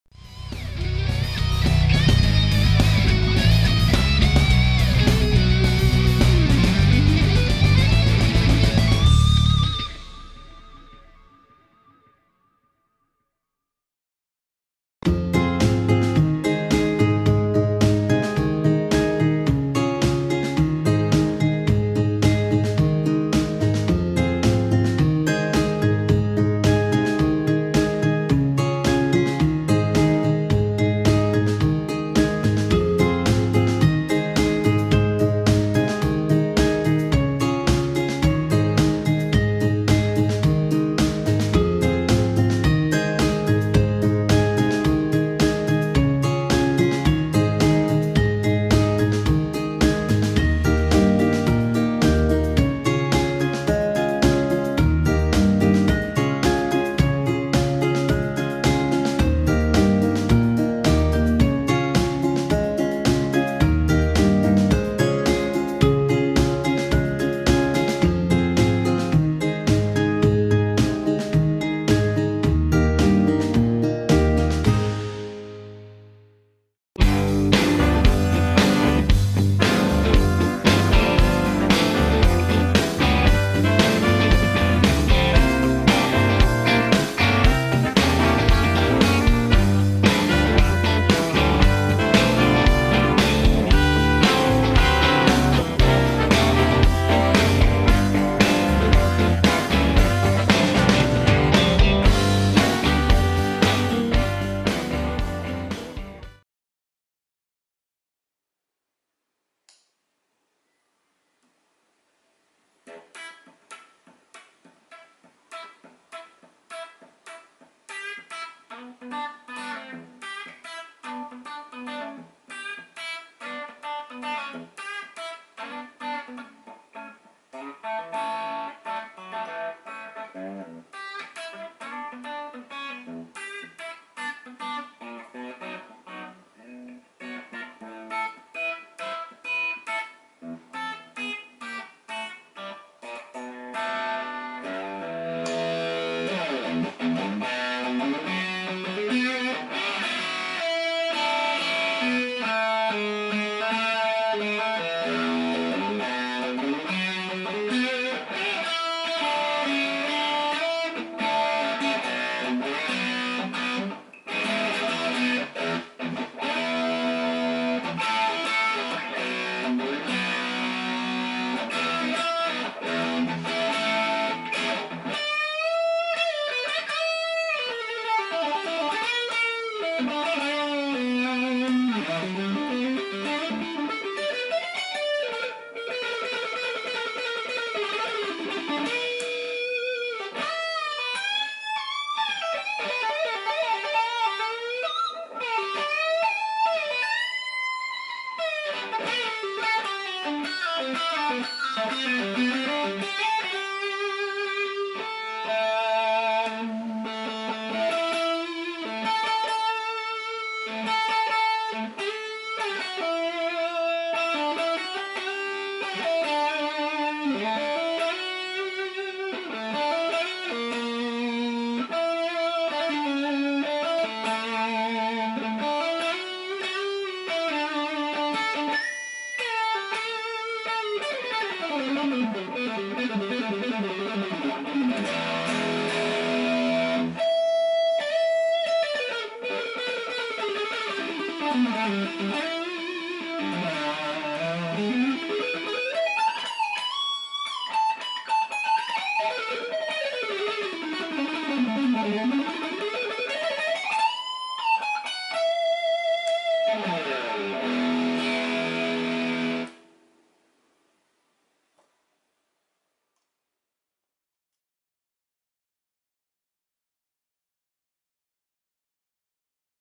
How to intonate your electric guitar
Sharing some basic knowledge to you, on how I intonate an electric guitar.

How I setup a guitar with a tremolo system